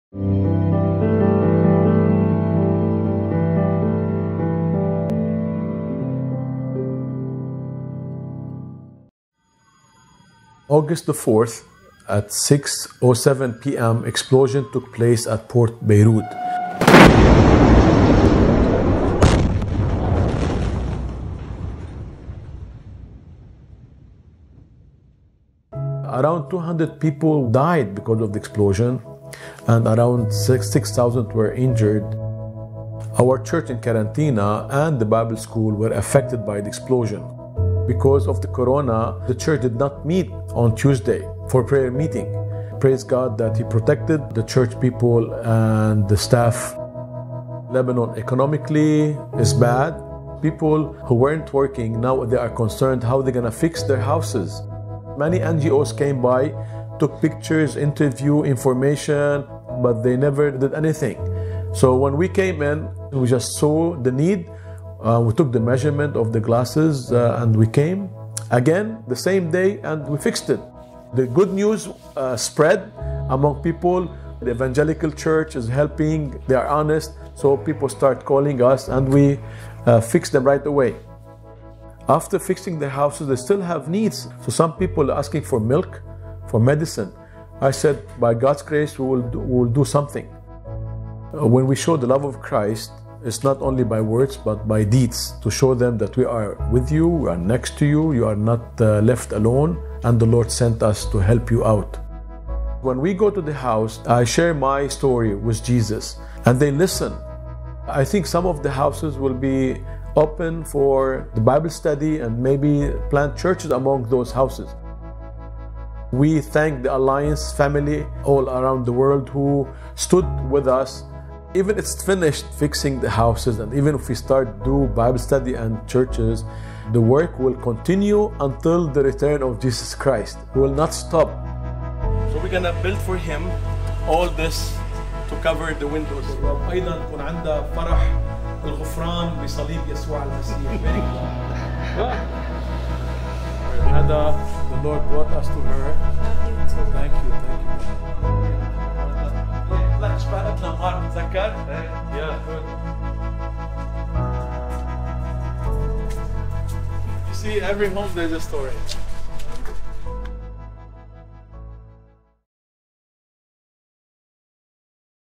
¡No Estás Solo!
La Alianza en Beirut comparte el amor de Cristo mientras ayuda a las personas afectadas por la explosión que tuvo lugar el 4 de agosto de 2020 a reconstruir sus hogares.

El Rev. Charbel Malak agradece a la familia de la Alianza en todo el mundo por todo su apoyo y oraciones.